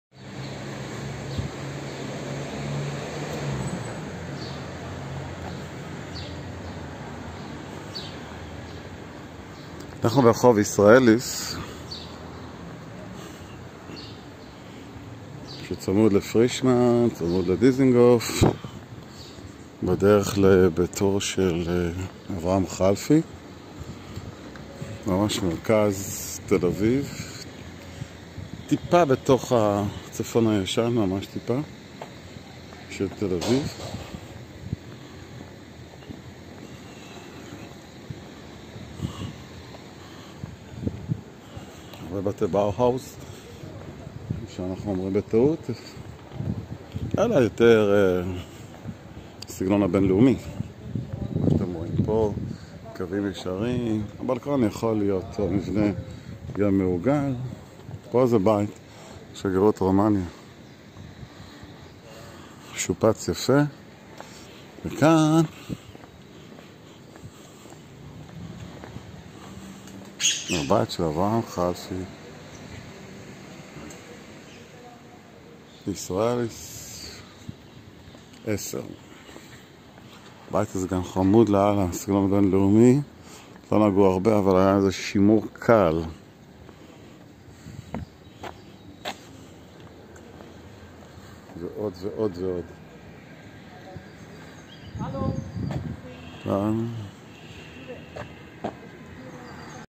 סיור ברחוב ישראליס תל אביב
סיור בעקבות אברהם חלפי ויצירותיו בתל אביב,סיור מודרך בתל אביב